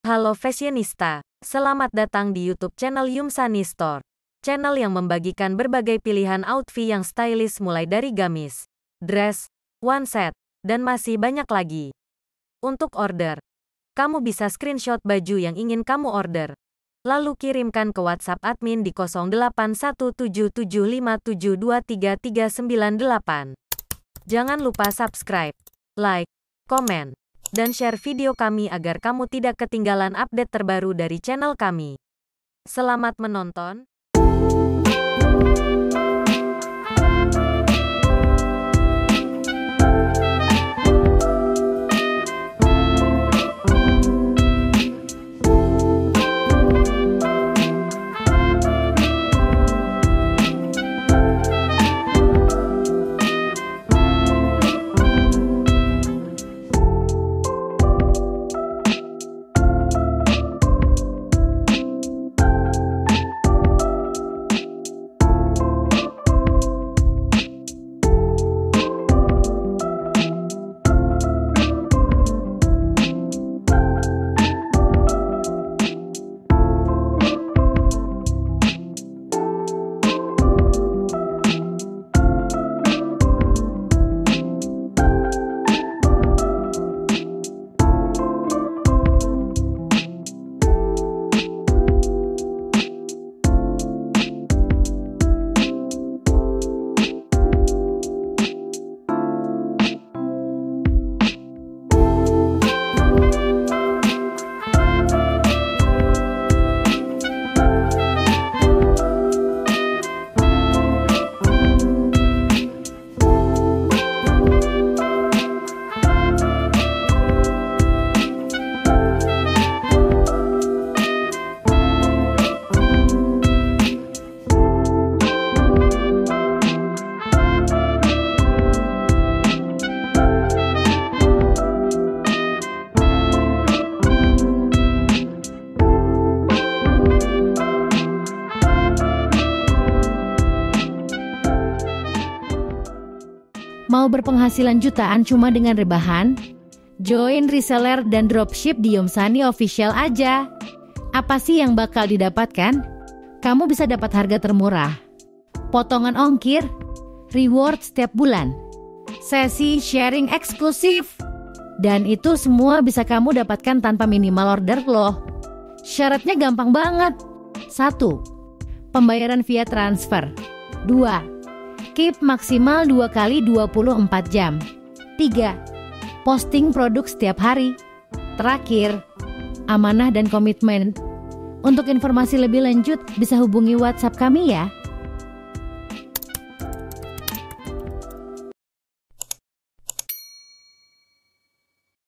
REFERENSI GAMIS DRESS SYARI MODERN DAN KEKINIAN 2024 // PART 4
Halo fashionista! 🛍️

Kalian pasti suka dengan pilihan outfit yang stylish tapi tetap budget-friendly, bukan? Nah, di video kali ini, Yumsani Store akan membagikan rekomendasi model gamis super cantik dengan harga hanya terjangkau! 😍

Lazada: Yumsani.store
Shopee: Yumsani.store
TikTokShop: Yumsani.store02

Yumsani Store